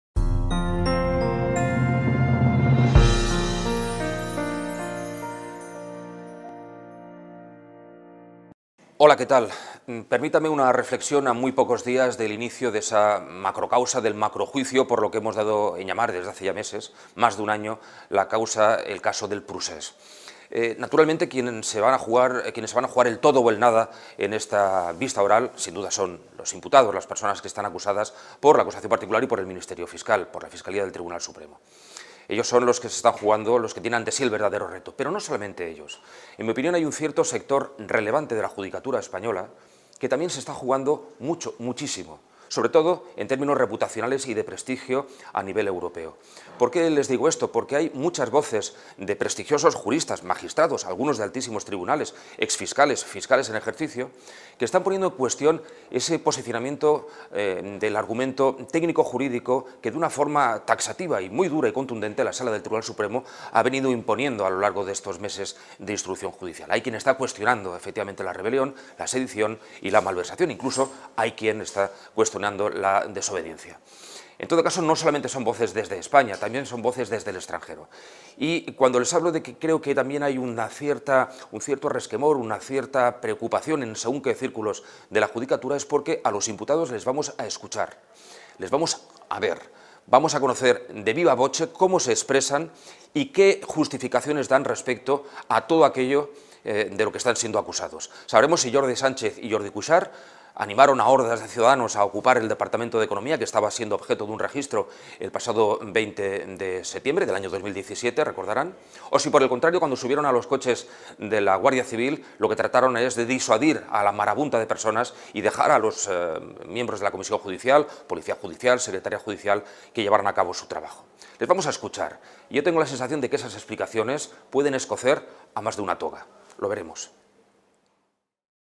Videoblog: jueces y acusados al descubierto en el juicio del "procés"
A principios del febrero se inicia el llamado juicio del "procés" y cada vez son más las voces, incluso del entorno judicial, que discrepan sobre los delitos de rebelión, sedición y malversación. Ahora ha llegado el momento de escuchar a los acusados.